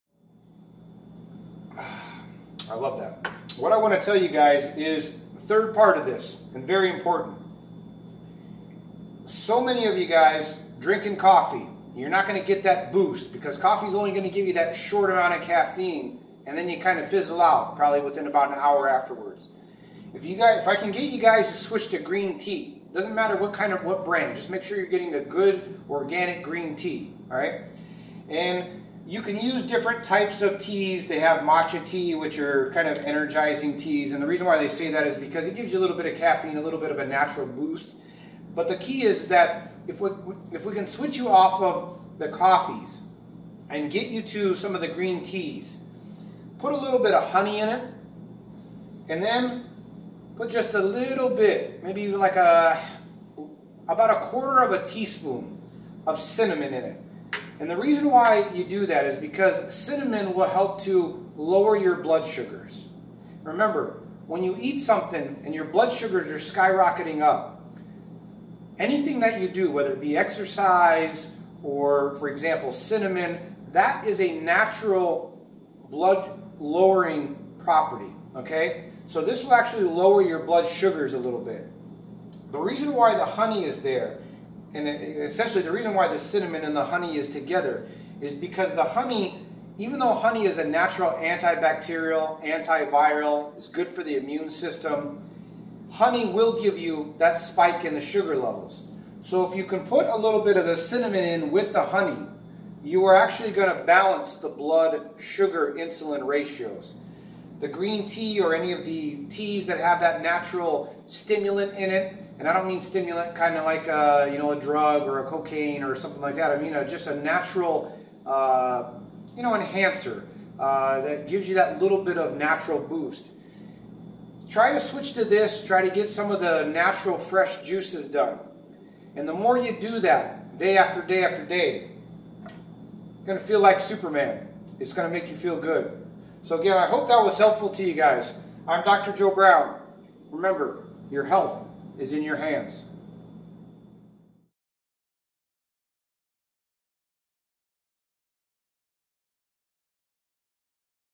Increase Energy and Cleanse Colon Juicing Part 3
Dr. Joe Brown talks about how to increase energy in your life and cleanse your colon by juicing these fruits and vegetables and why they are important to your everyday natural health.

Part 3/3